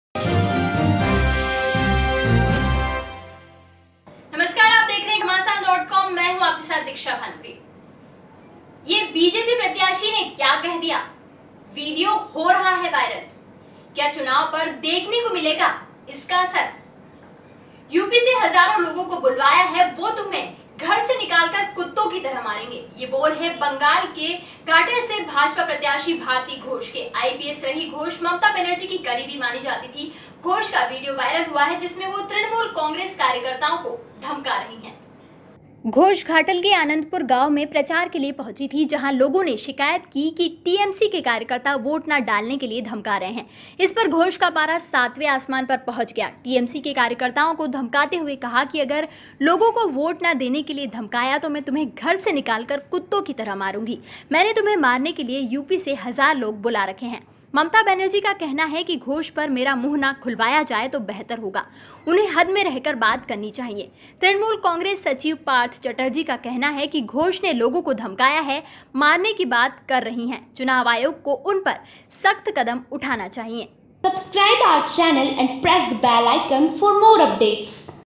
गुस्साई BJP नेता Bharti Ghosh ने क्या कह दिया
'UP has called thousands of people. He will take you out of the house and kill them like dogs. It is said that Bharti Ghosh, the BJP candidate from Bengal Ghatal. Ghosh, who was an IPS officer, was considered close to Mamta Banerjee. Ghosh's video has become viral, in which he is threatening the Trinamool Congress workers.
Ghosh, reached Ghantal in Anandpur village, where people complained that the workers of TMC are threatening to cast votes. On this, Ghosh's mercury reached the seventh heaven. Threatening the workers of TMC, he said that if people threaten to not vote, then I will take you out of the house as dogs. I have called thousands of people from UP to kill you. Mamta Banerjee says that it would be better if I do not open my mouth on Ghosh. They should talk and stay in the extent. Trinamool Congress Secretary Partha Chatterjee says that Ghosh has threatened people. Talking to kill Election Commission should take strict action against them.